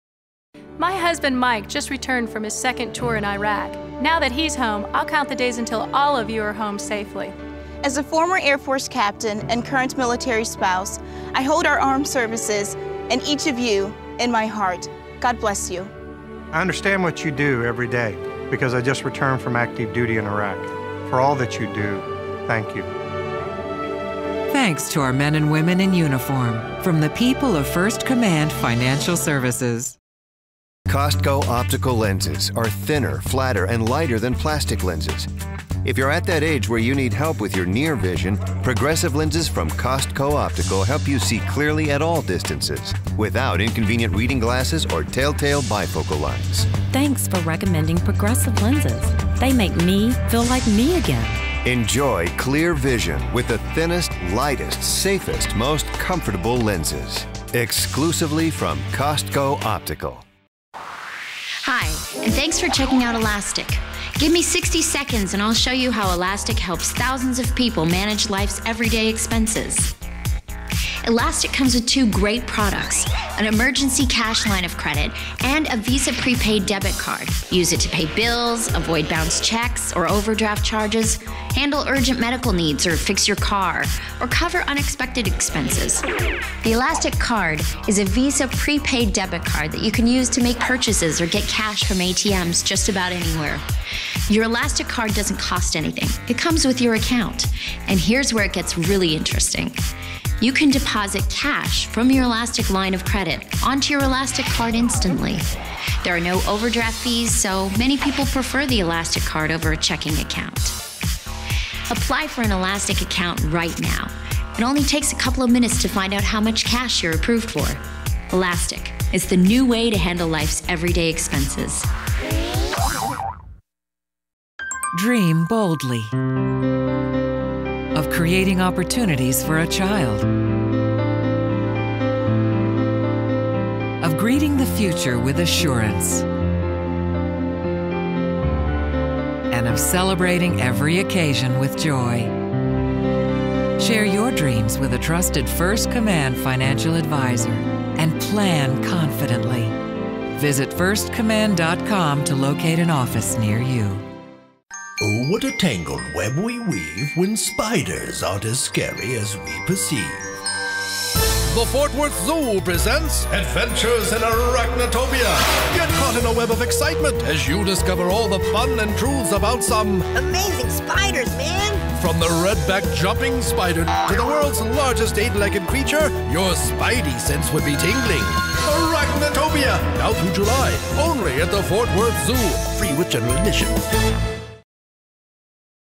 Broken Films Commercials Cable Non Broadcast Reel
Regional, cable and non-broadcast spots for agency and corporate clients. National commercial demo on request.